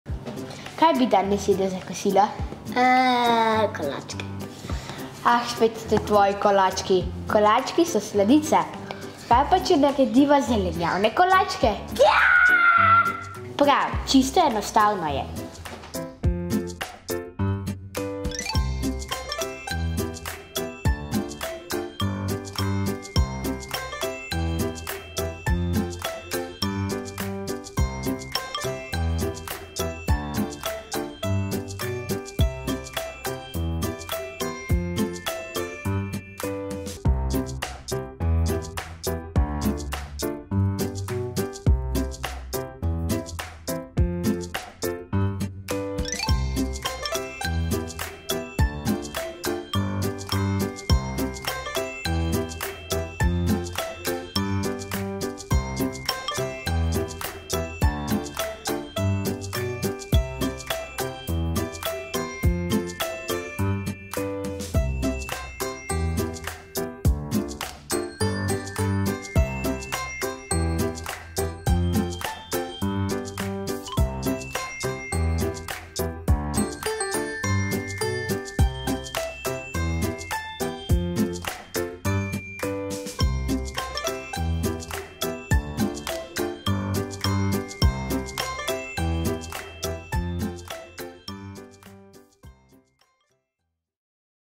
Zdravko Lidl: Zelenjavni kolački
Če je tvoj otrok takšen ljubitelj kolačkov, da jih želi tudi za kosilo, mu pripravi preproste zelenjavne kolačke. 😋 Katero jed pa tvoj otrok naravnost obožuje? 👇

Vsebine Zdravka Lidla so na voljo vsak 1. teden v mesecu.

0:00 Uvod
0:22 Priprava kolačkov
1:16 Končna jed
1:28 End screen